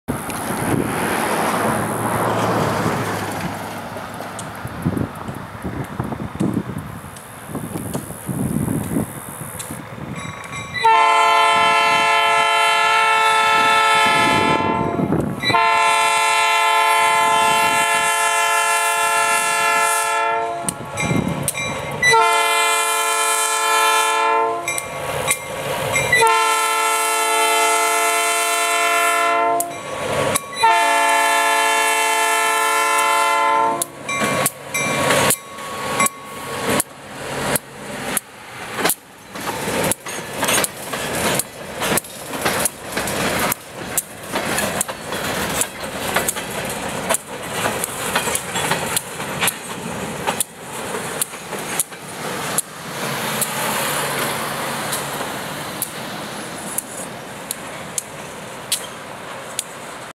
Tulsa-Sapulpa Union Railway just outside of Sapulpa,Ok
The train now has 2 grain hoppers and is heading towards Tulsa.